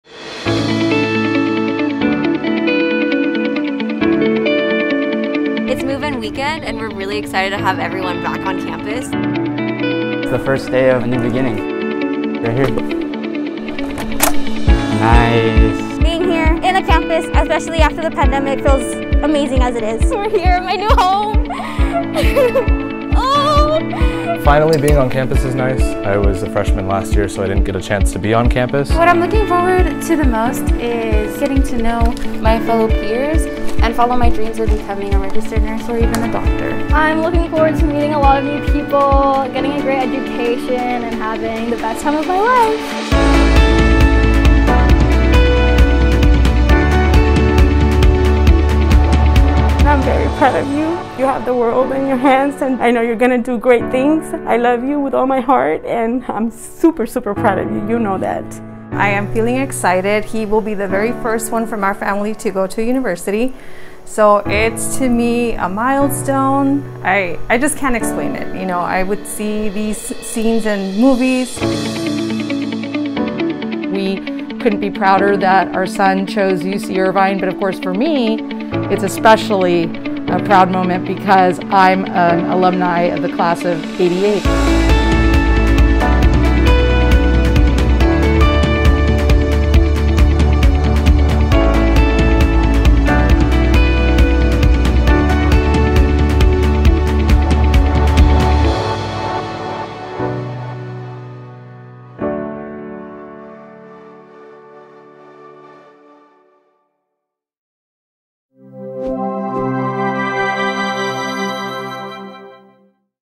Move-In 2021 - UC Irvine
UCI welcomed students moving in, which includes undergraduate, graduate, transfer and returning students.

Video by Steve Chang/UCI